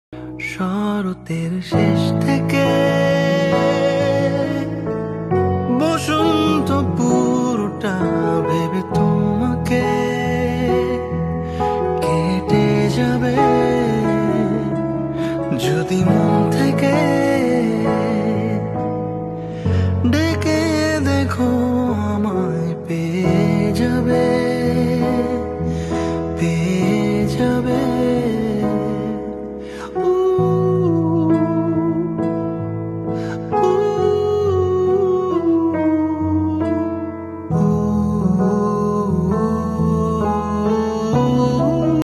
শরতের শেষ থেকে/ তারাকান্দি, সরিষাবাড়ী, জামালপুর।
শরতের দিনের ভিডিও । তারাকান্দি সরিষাবাড়ী থেকে ভিডিও টা করা হয়।

✅ Google Map Location: sarishabari, jamalpur, tarakandi

YouTube Channel name:
Singer & Songwriter-
Music Producer- Bryce
Dop-